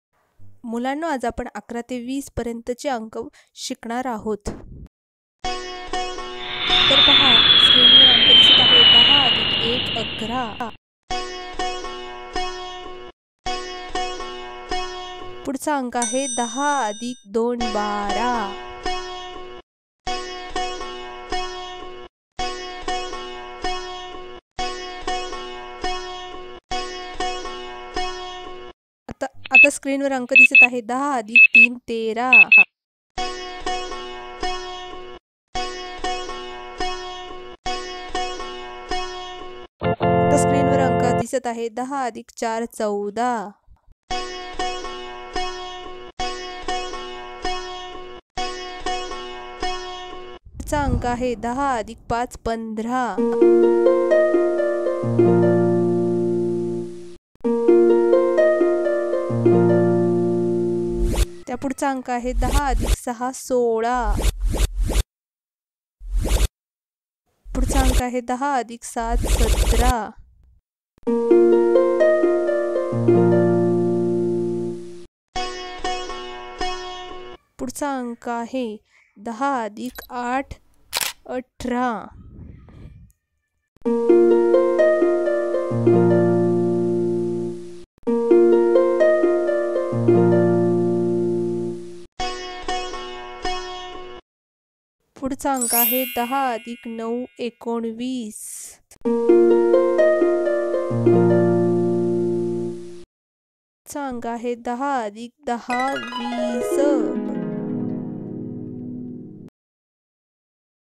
११ते २० पर्यंत अंकाचा सराव असा घ्या मुले कधीच विसरणार नाहीत
This vedio is all about11 to 20 numbers. How to write and read 11 to 20